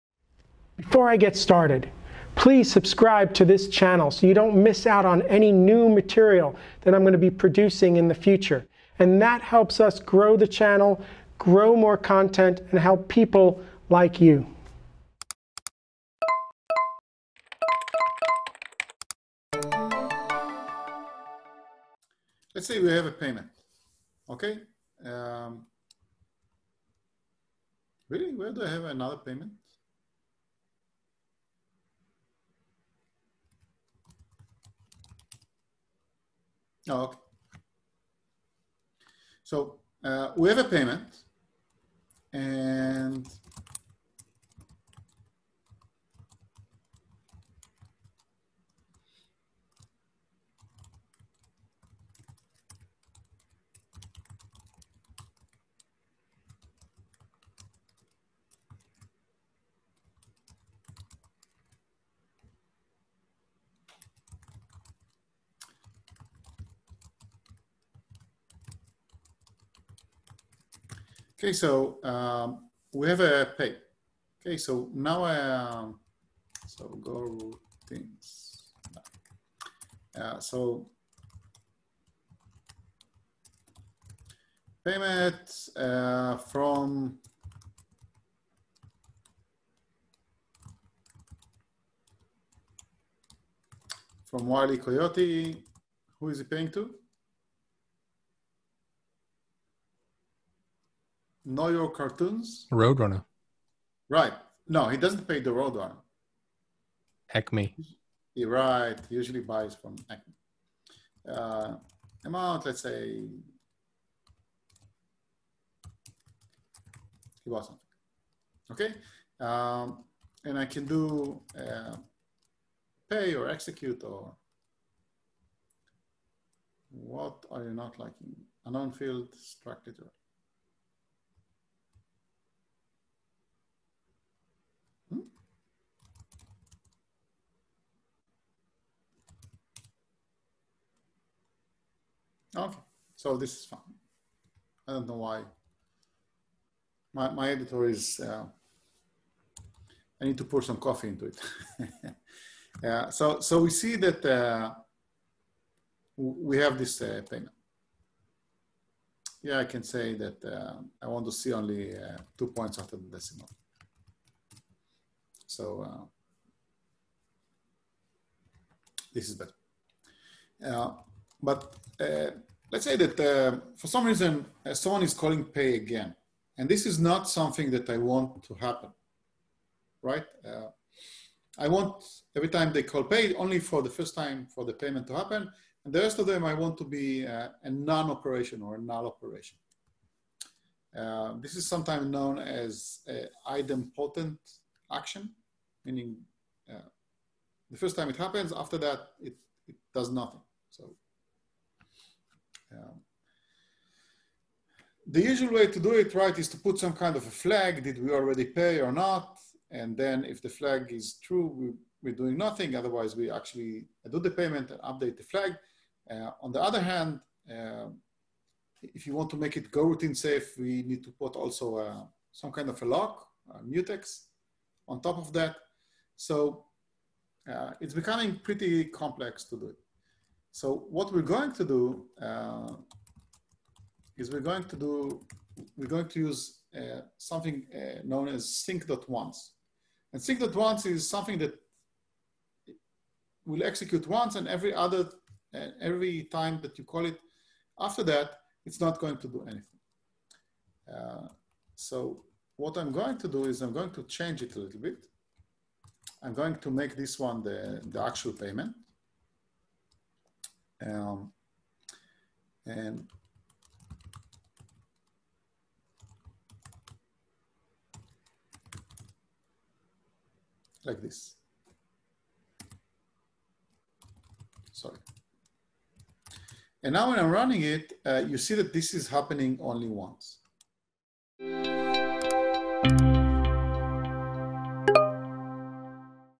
Using sync.Once for idempotency
In this example, we want to run a payment and limit it to running only once. This is something called idempotent action, meaning it will execute once and will do nothing if called again.